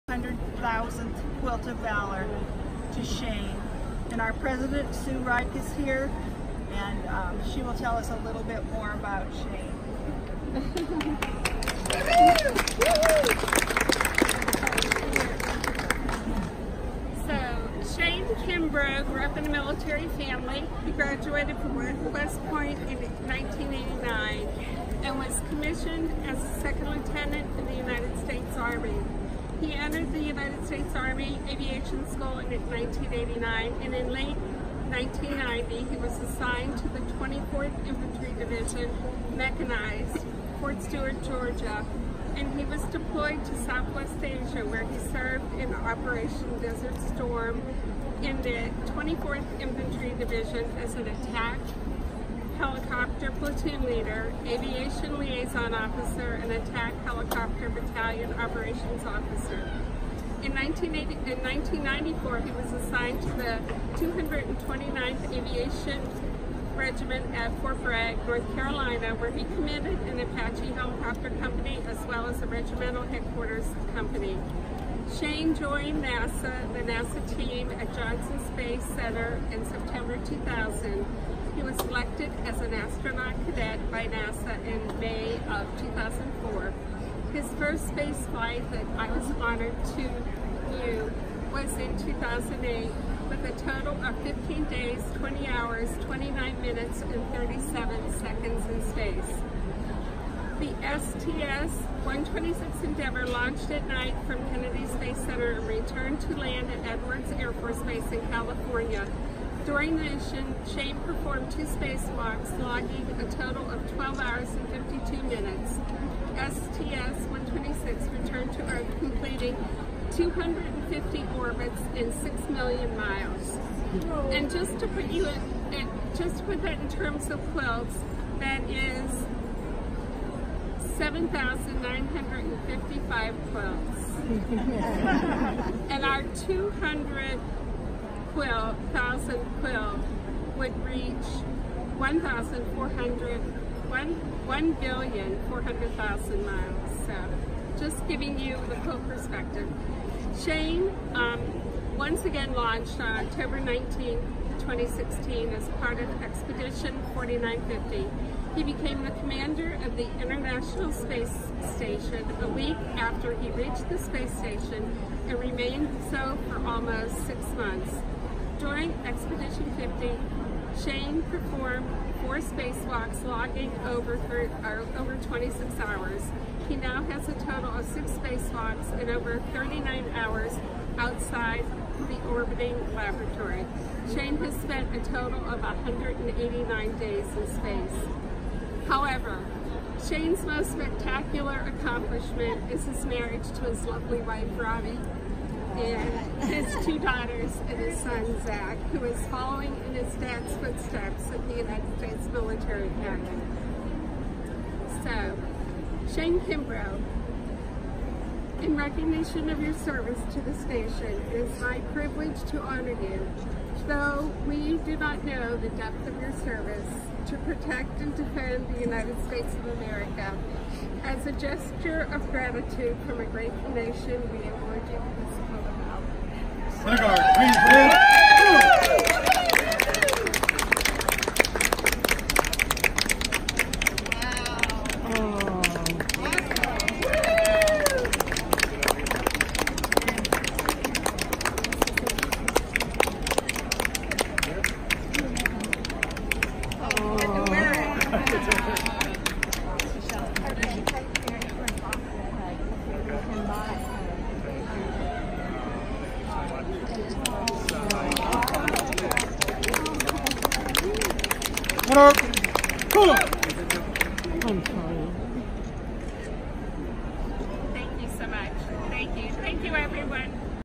Quilt of Valor Presentation 2018 Houston Quilt Festival
Astronaut Shane Kimbrough is presented with the 200,000th Quilt of Valor.